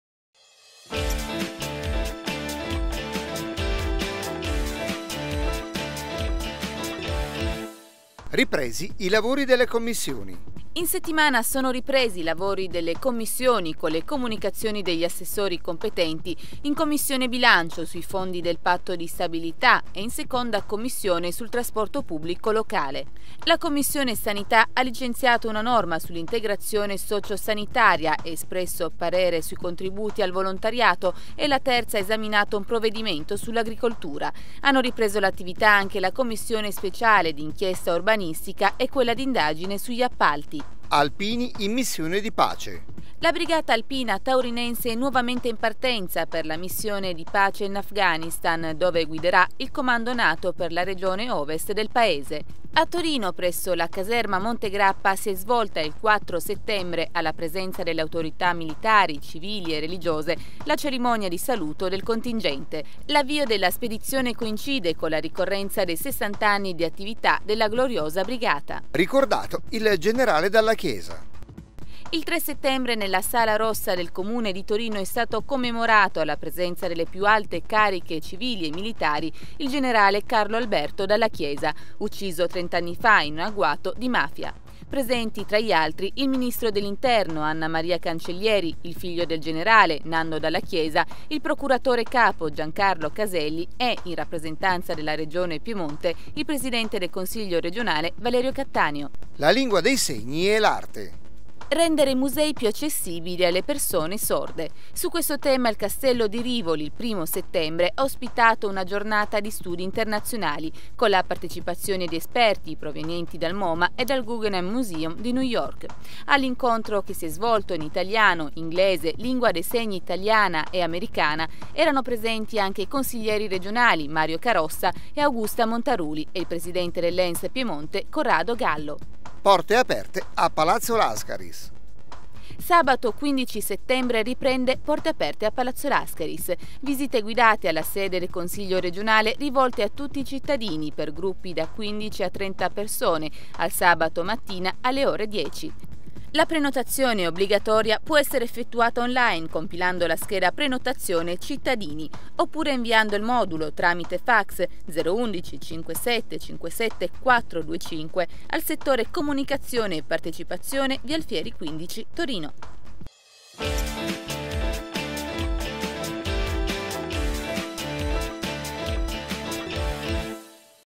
Crp-Webtg n.36 del 7 settembre 2012
Ripresi i lavori delle Commissioni
In settimana sono ripresi i lavori delle Commissioni con le comunicazioni degli assessori compenti, in Commissione Bilancio sui fondi del Patto di stabilità e in seconda Commissione sul trasporto pubblico locale. La Commissione Sanità ha licenziato una norma sull'integrazione socio-sanitaria e espresso parere sui contributi al volontariato e la terza ha esaminato un provvedimento sull'agricoltura. Hanno ripreso l'attività anche la Commissione speciale d'inchiesta urbanistica e quella d'indagine sugli appalti.

Alpini in missione di pace
La Brigata Alpina Taurinense è nuovamente in partenza per la missione di pace in Afghanistan, dove guiderà il Comando Nato per la regione ovest del paese. A Torino, presso la Caserma Montegrappa, si è svolta, il 4 settembre, alla presenza delle autorità militari, civili e religiose, la cerimonia di saluto del contingente. L'avvio della spedizione coincide con la ricorrenza dei sessant'anni di attività della gloriosa Brigata.

Ricordato il generale Dalla Chiesa
Il 3 settembre nella Sala Rossa del Comune di Torino è stato commemorato, alla presenza delle più alte cariche civili e militari, il generale Carlo Alberto Dalla Chiesa, ucciso 30 anni fa in un agguato di mafia. Presenti, tra gli altri, il ministro dell'Interno, Anna Maria Cancellieri, il figlio del generale, Nando Dalla Chiesa, il procuratore capo Giancarlo Caselli e, in rappresentanza della Regione Piemonte, il presidente del Consiglio regionale Valerio Cattaneo.

La Lingua dei segni e l'arte
Rendere i musei più accessibili alle persone sorde. Su questo tema il Castello di Rivoli, il primo settembre, ha ospitato una giornata di studi internazionali, con la partecipazione di esperti provenienti dal Moma e dal Guggenheim Museum di New York. All'incontro, che si è svolto in italiano, inglese, lingua dei segni italiana e americana, erano presenti anche i consiglieri regionali Mario Carossa e Augusta Montaruli e il presidente dell'Ens Piemonte, Corrado Gallo.

Porte aperte a Palazzo Lascaris
Sabato 15 settembre riprende "Porte aperte a Palazzo Lascaris": visite guidate alla sede del Consiglio regionale, rivolte a tutti i cittadini, per gruppi da 15 a 30 persone, al sabato mattina alle ore 10. La prenotazione, obbligatoria, può essere effettuata on-line compilando la scheda prenotazione cittadini, oppure inviando il modulo tramite fax (011/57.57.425) al Settore Comunicazione e Partecipazione, via Alfieri 15, 10121 Torino.